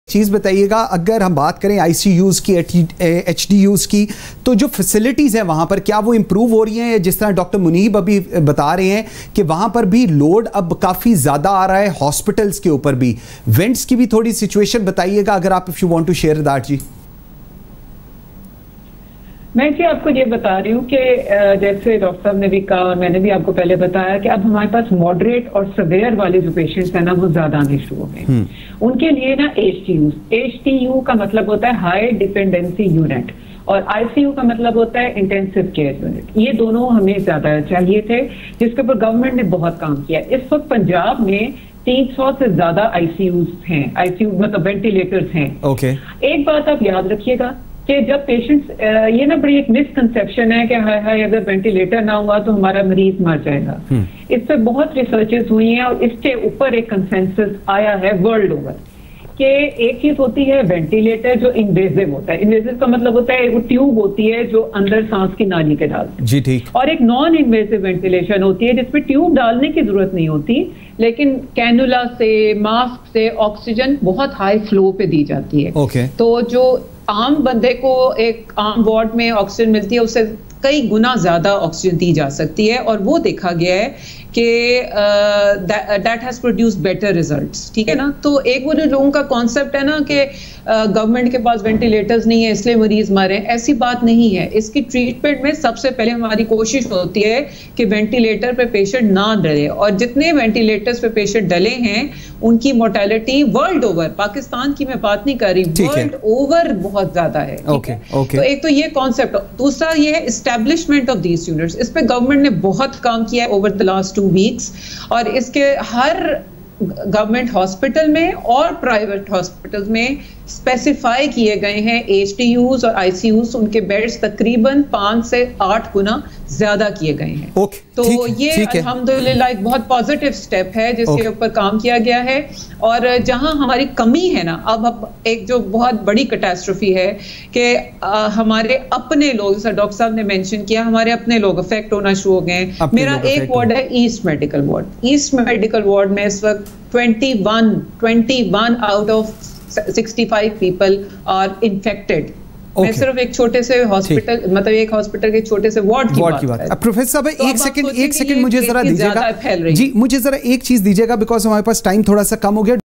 Ek Choty say Hospital Mai 21 Doctors Corona Se Mutasir Hogay : Dr. Bilquis Shabbir
Ek Choty Hospital Mai 21 Doctors Corona Se Mutasir Hogay : Dr. Bilquis Shabbir